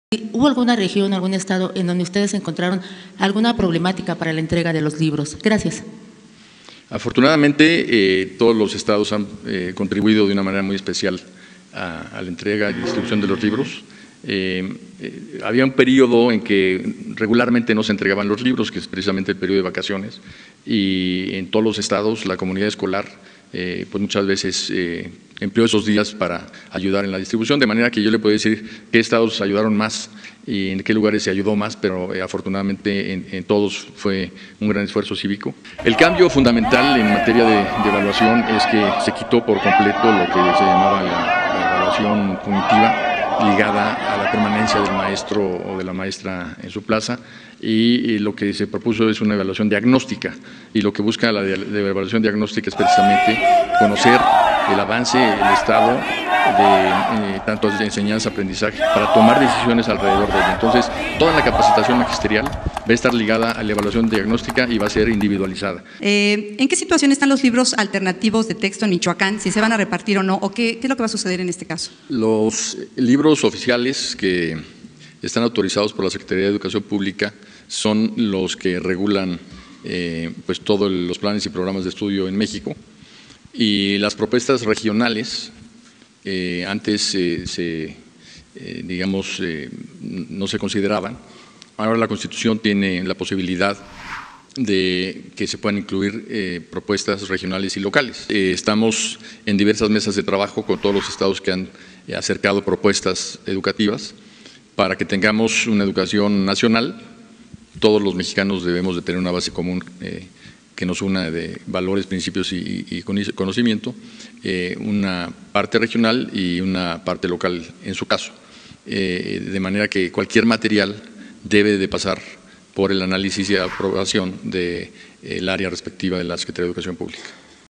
Sobre los libros de texto para el nuevo ciclo escolar
La conferencia se centró en los libros de texto, donde Esteban Moctezuma niega que hayan tenido problemas para entregar los libros, también habla sobre la evaluación diagnóstica de los maestros y señaló la posibilidad de que haya libros regionales como los de La CNTE en Michoacán 😬